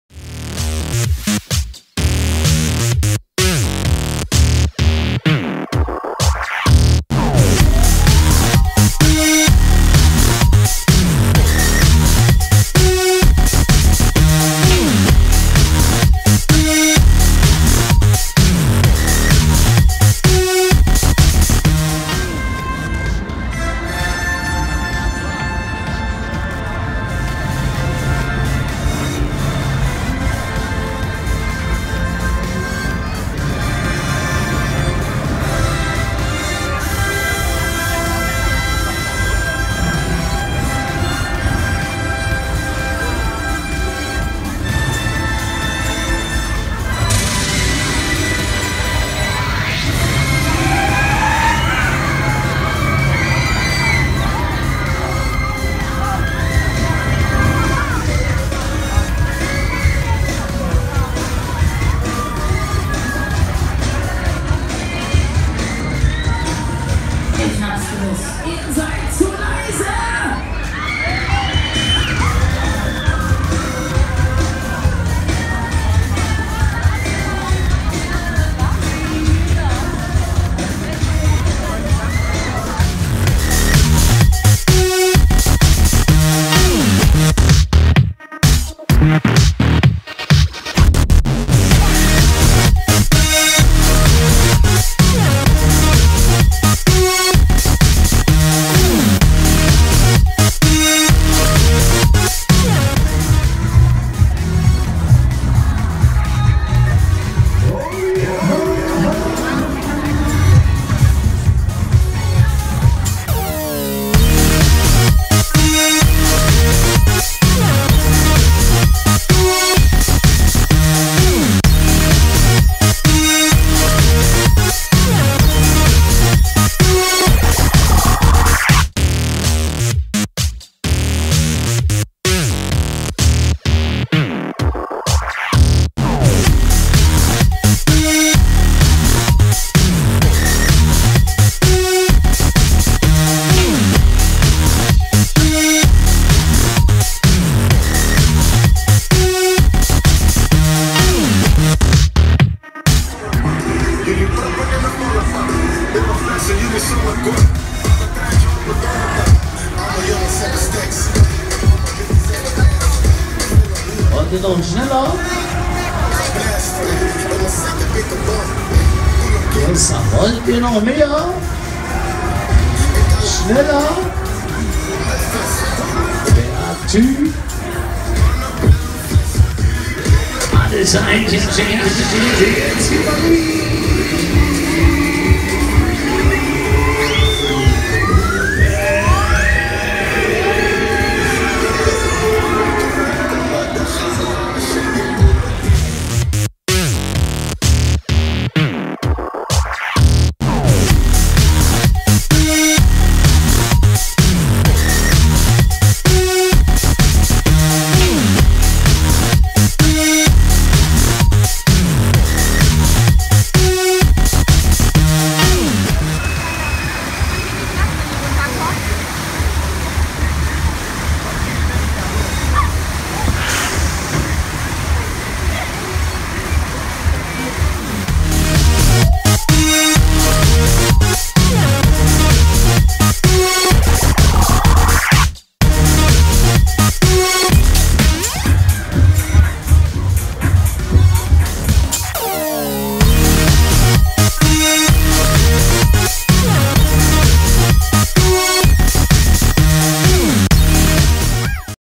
Stoppelmarkt Vechta 2018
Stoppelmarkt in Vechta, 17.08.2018
Die interessantesten Fahrgeschäfte.
War auf jeden Fall die längere Anreise wert,nächstes Jahr wieder!